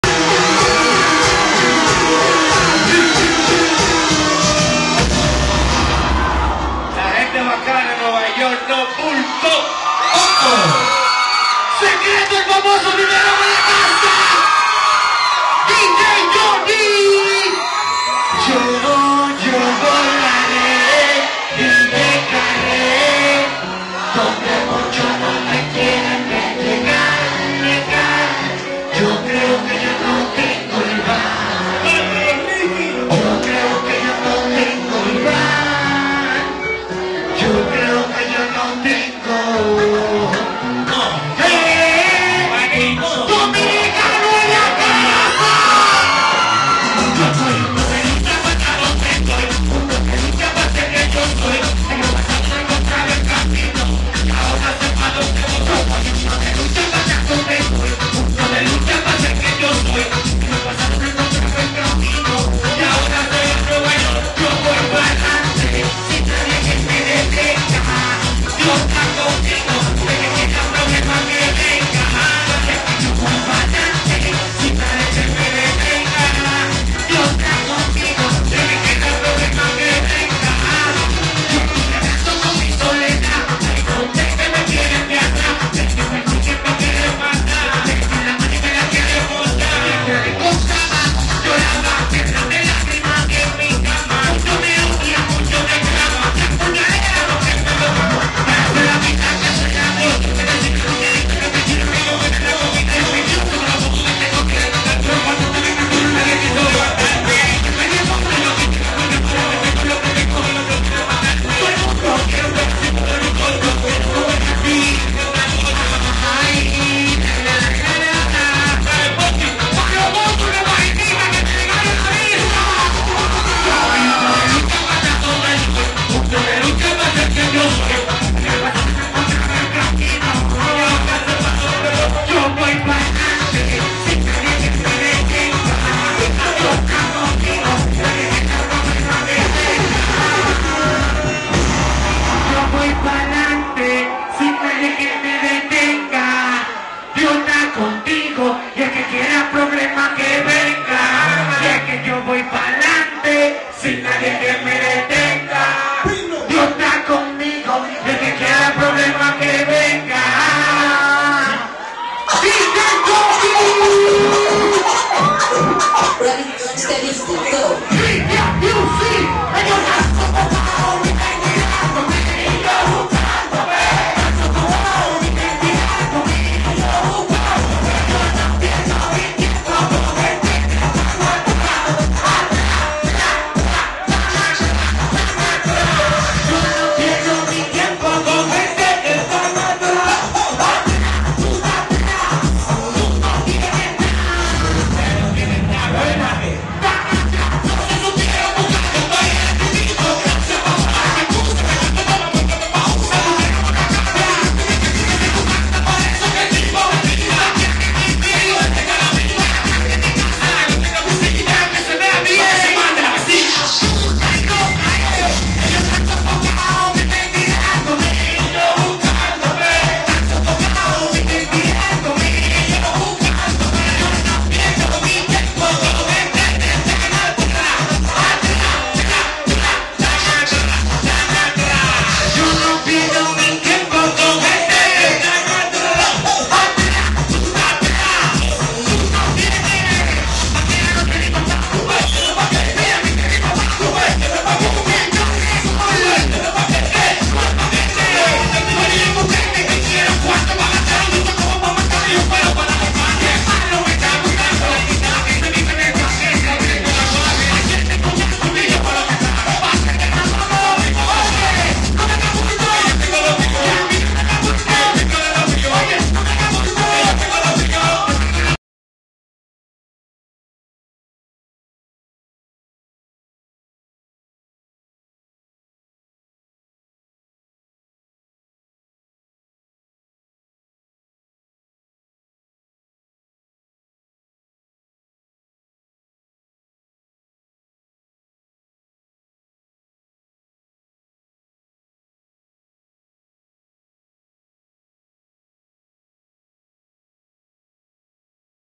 SECRETO EN VIVO EN SAL Y PIMIENTA ::: NOV. 29, 2011 TUESDAY
RAW FOOTAGE OF SECRETO EL MAS FAMOSO BIBERON. LIVE AT SAL Y PIMIENTO TUESDAY NOV. 29TH 2011.